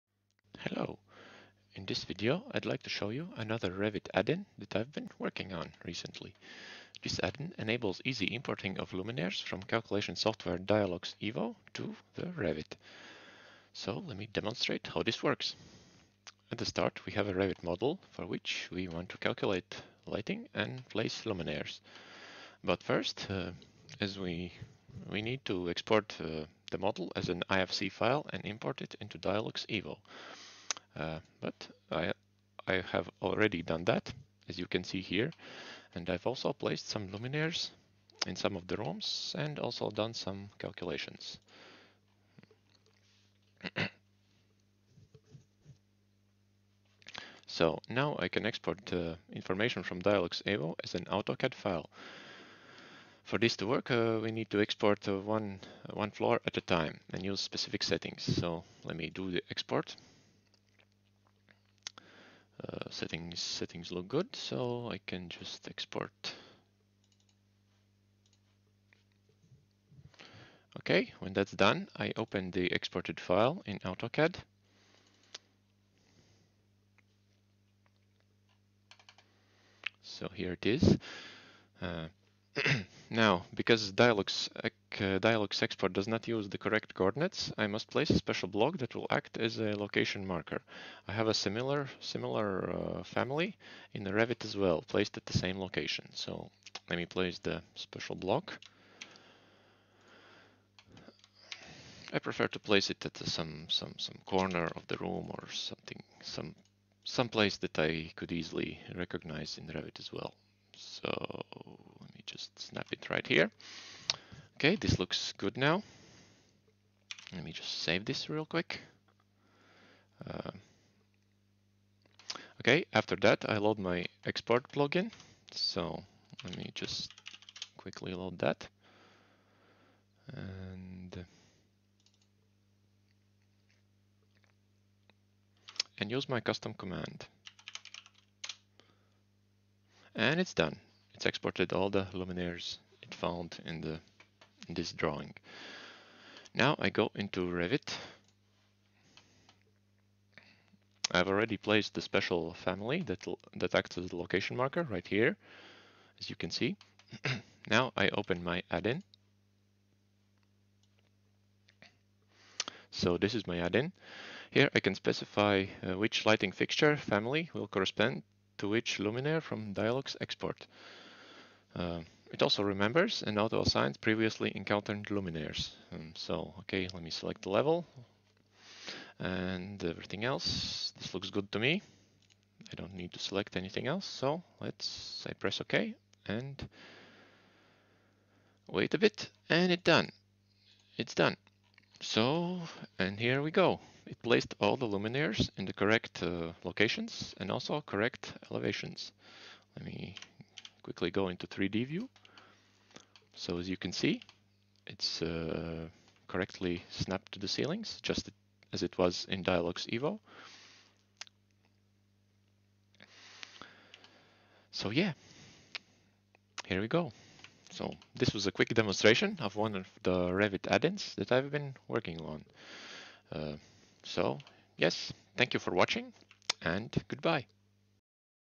Revit Automation. Custom add-in demo (Dialux EVO to Revit import)
A quick demonstration for one of the Revit add-ins, that I've been developing.
This add-in enables easy importing of luminaires from the calculation software Dialux Evo to Revit.
Made with C#.net, Revit API and UI in WPF/MVVM.